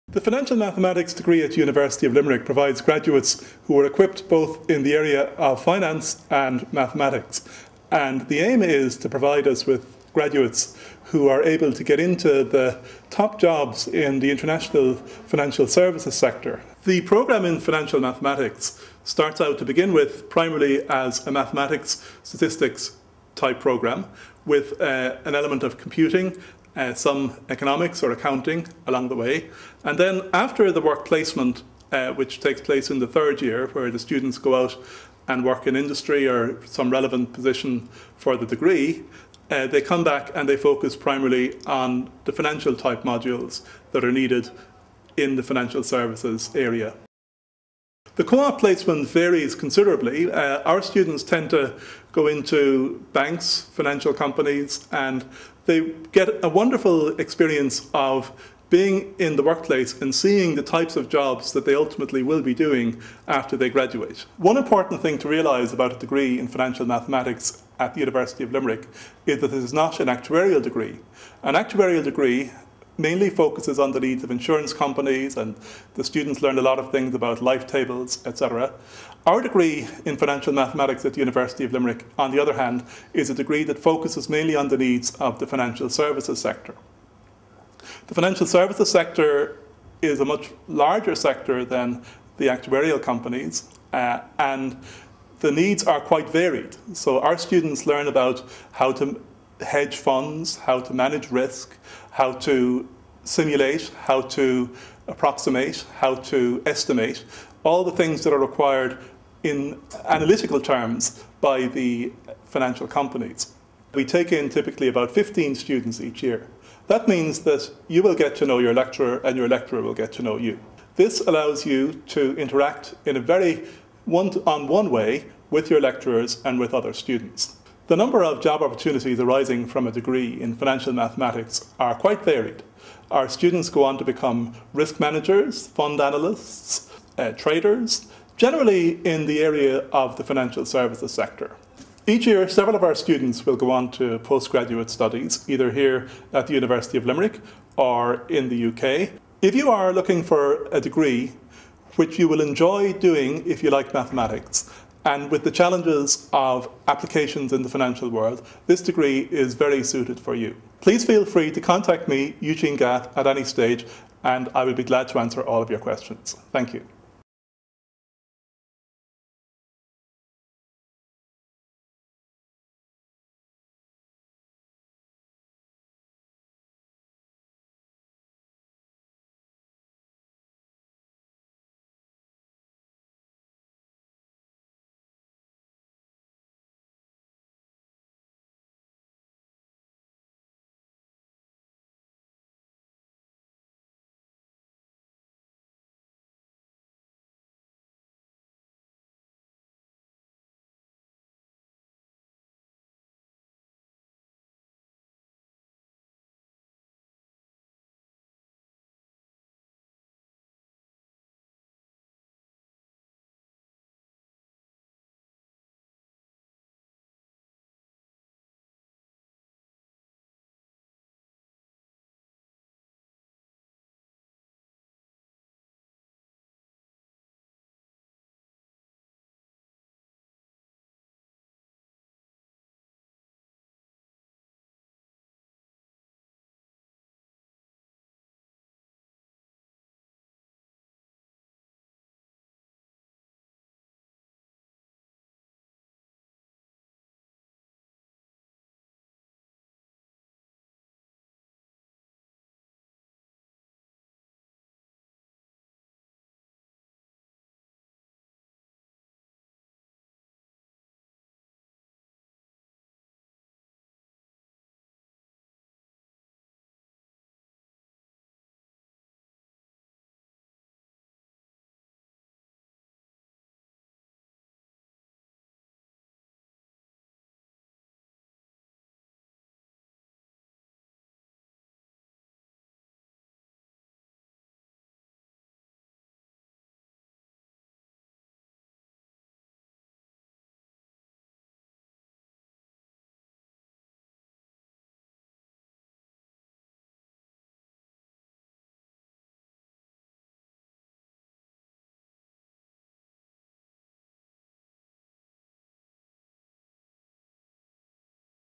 Course Leader Bachelor of Science in Financial Mathematics at the University of Limerick LM058
Financial Maths is a great choice for someone who like maths and wants to work in the business, financial or banking sectors. Business degrees tend to have very little maths, so most employers are very keen to hire graduates with good quantitative skills as well a financial background. Students learn about trading, asset management and risk analysis of complicated financial products while at the same time developing solid mathematical skills.There are many career opportunities in financial trading companies, financial exchanges, investment companies, banks, government financial bodies, insurance companies, energy and power companies, agribusinesses etc.

The aims of the degree are to produce graduates with developed mathematical, statistical and computing skills, and the ability to apply them to the quantitative analysis of industrial, commercial or financial business decisions. The programme also aims to produce graduates with sufficient mathematical, statistical and computing skills for them to undertake postgraduate work in these or related areas.